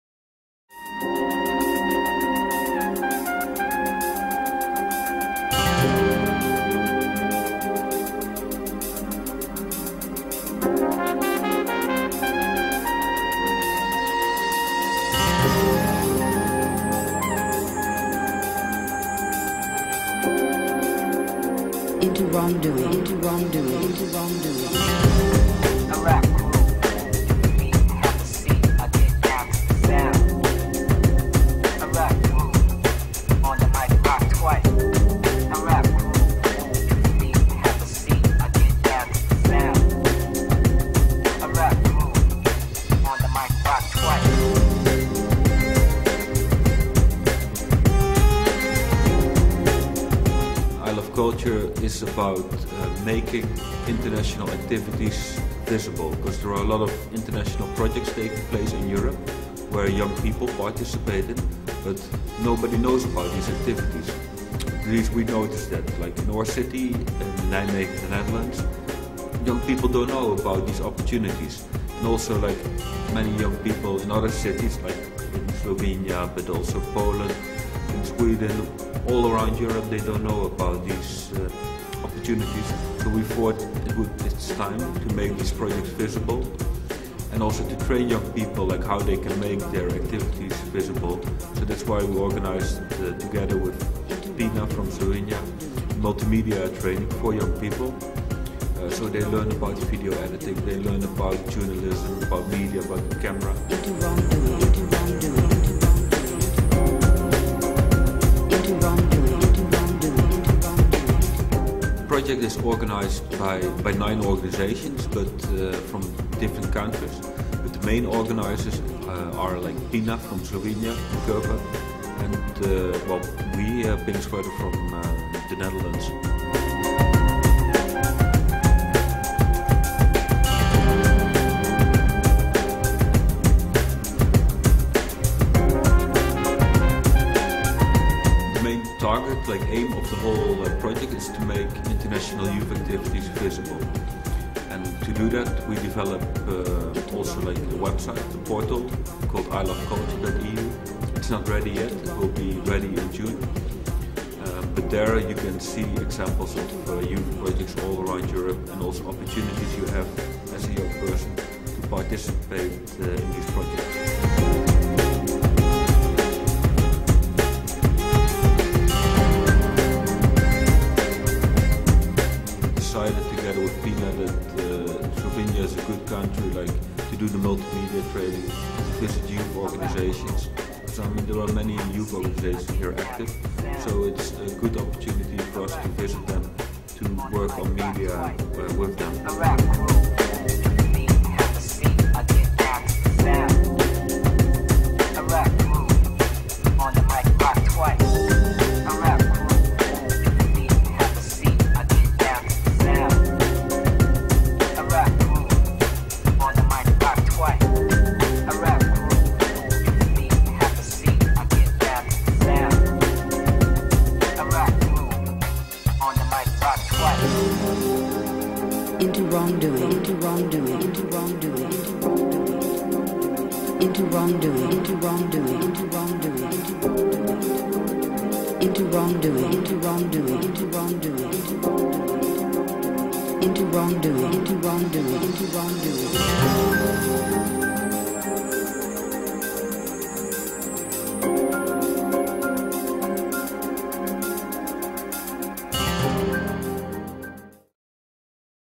ILoveCulture.EU
"iloveculture.eu", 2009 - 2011 yılları arasında Avrupa'dan 60 STK'nın ve 5.000 gencin katılımıyla Action 4.5 Programı çerçevesinde gerçekleştirdiğimiz "Gençlik Eylemde Kültür Portalı projesidir. Web sitemizi ziyaret ederek bilgi alabilirsiniz:
"iloveculture.eu" is our "Youth in Action Cultural Portal" Action 4.5 Informative Project, realized from 2009 to 2011, involving 60 NGOs and 5.000 youngsters in Europe.